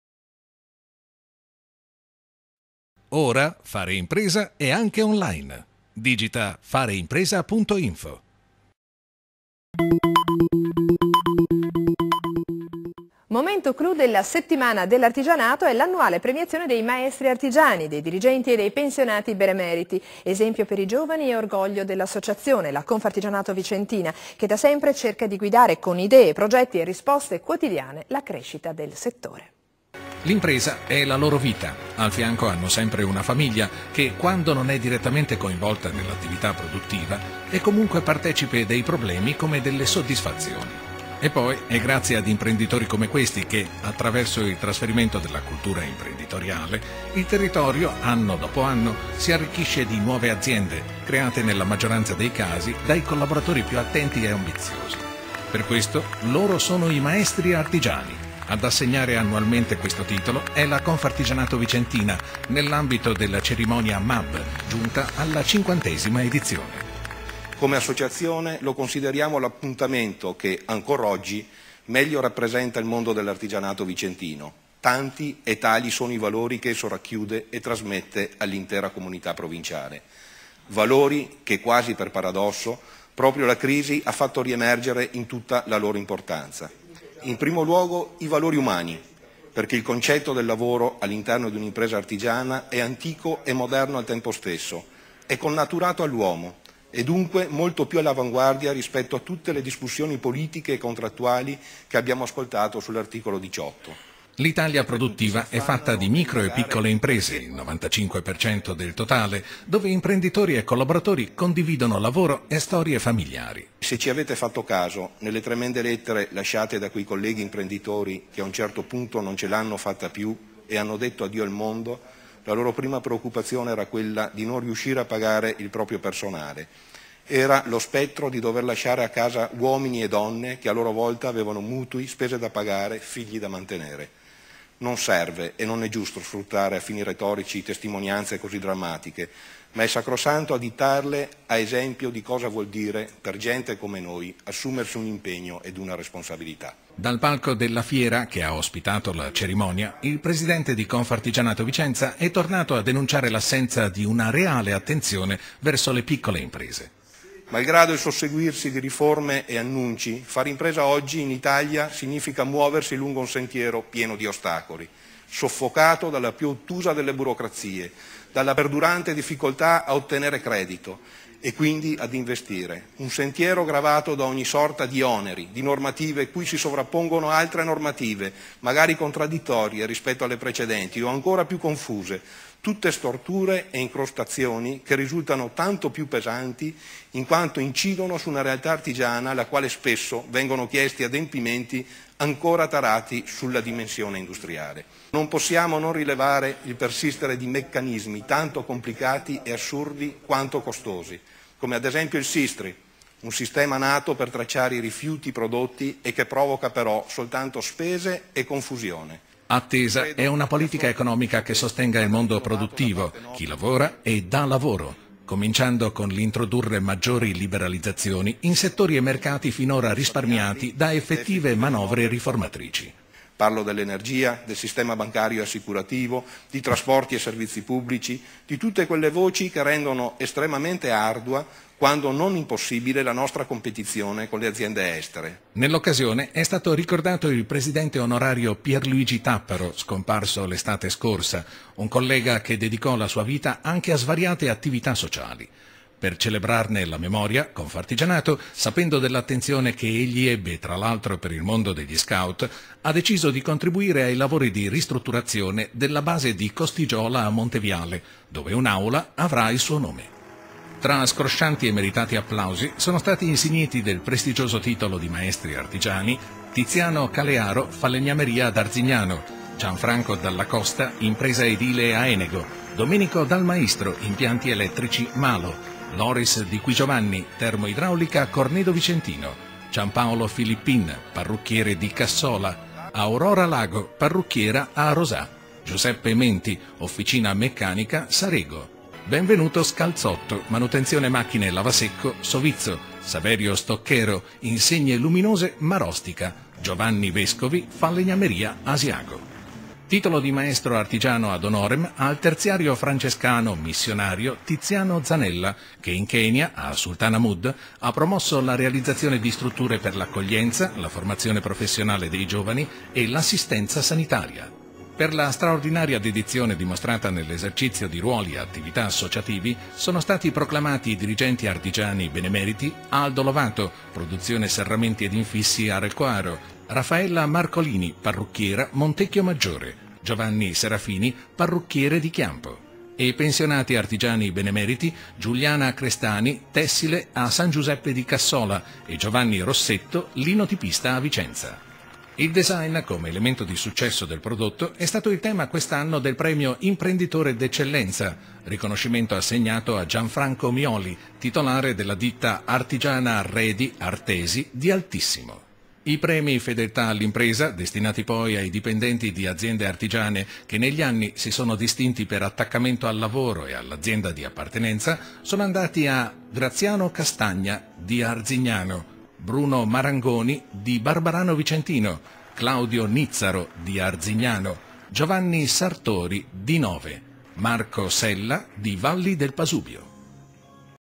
Maestri Artigiani divulgatori della cultura d'impresa
L'impresa è la loro vita ed è grazie ad essi che, attraverso il trasferimento della cultura imprenditoriale, il territorio, anno dopo anno, si arricchisce di nuove aziende. Sono i "Maestri Artigiani". Ad assegnare questo titolo è la Confartigianato vicentina, nell'ambito della cerimonia MAB, giunta alla cinquantesima edizione.